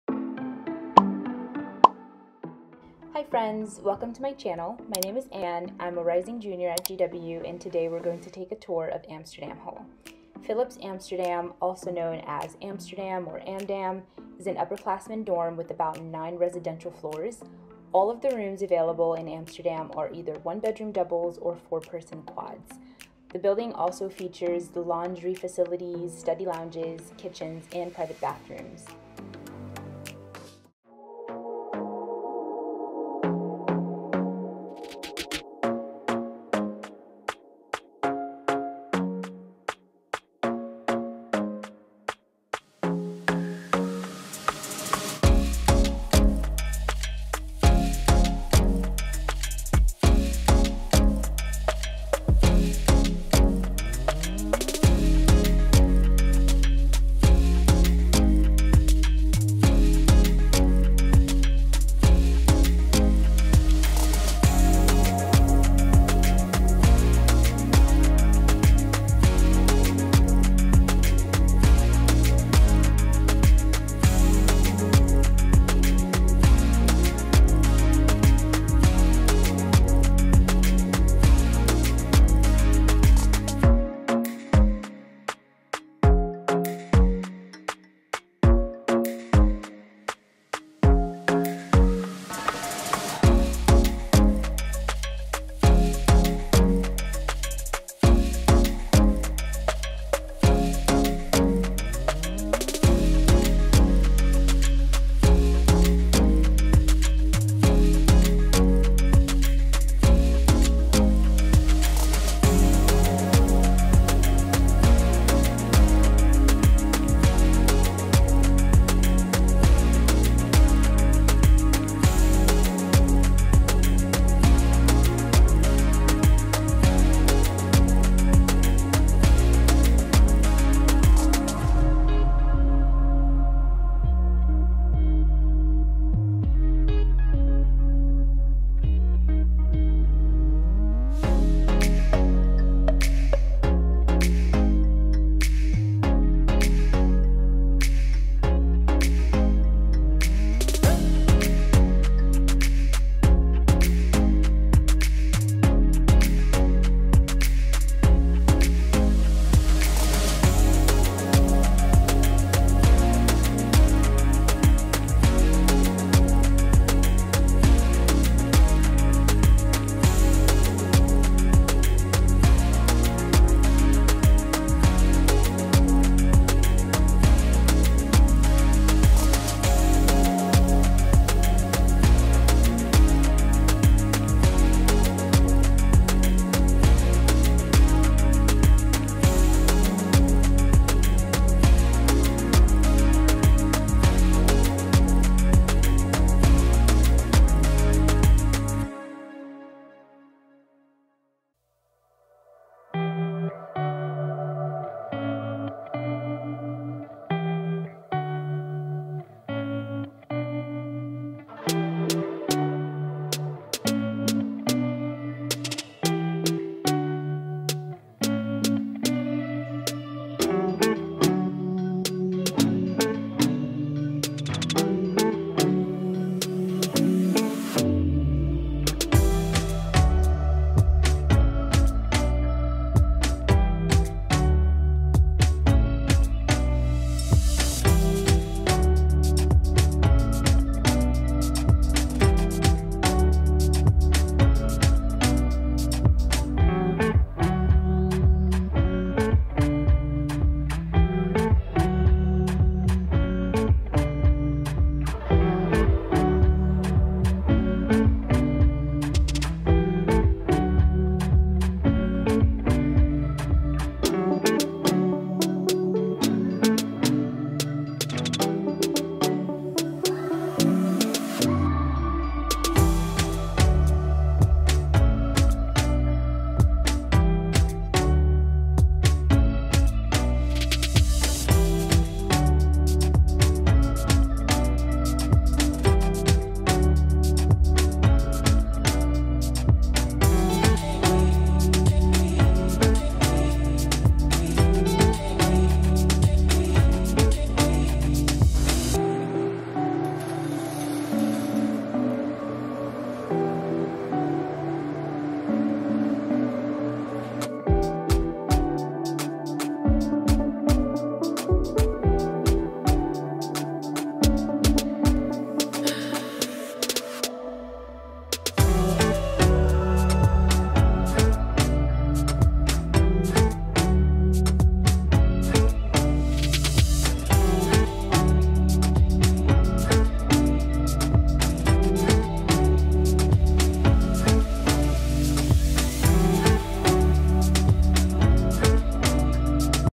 Amsterdam Hall Dorm Tour| George Washington University dorm tour| GWU dorm tour series
Hey Friends! Welcome to this week's video! Today we are touring a dorm in Amsterdam Hall at the George Washington University. We're going to see the ground floor where the laundry room, study spaces, and mail room is. We're also going to see a four person quad, with a full kitchen, a living room, two bedrooms, and two private bathrooms. Let me know down in the comments if you have any questions, and leave some recs for what dorms you'd like to tour next!

Shoutout to GW Res. Life for letting us tour this hall!

✰let's be friend on IG